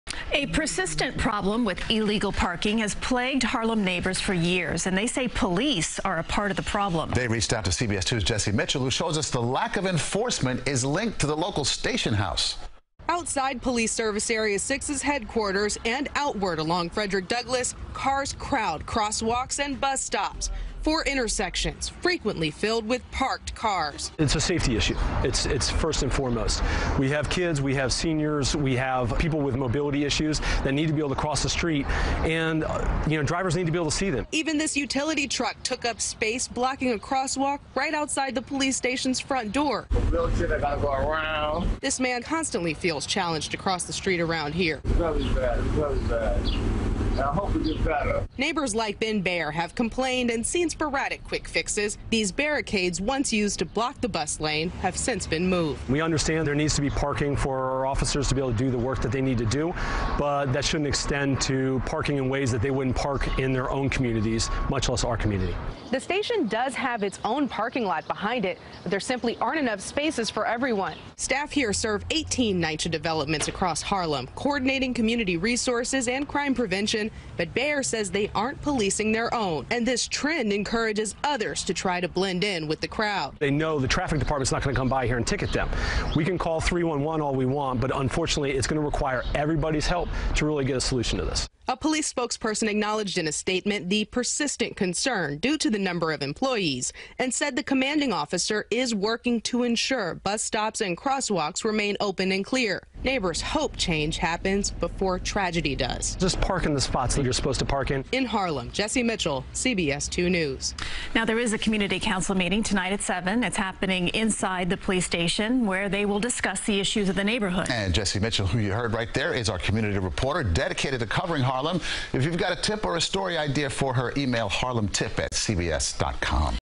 Parking problems persist outside Harlem police station
A persistent problem with illegal parking has plagued Harlem neighbors for years, and they say police are a part of the problem. They reached out to CBS2's Jessi Mitchell, who shows us the lack of enforcement is linked to the local station.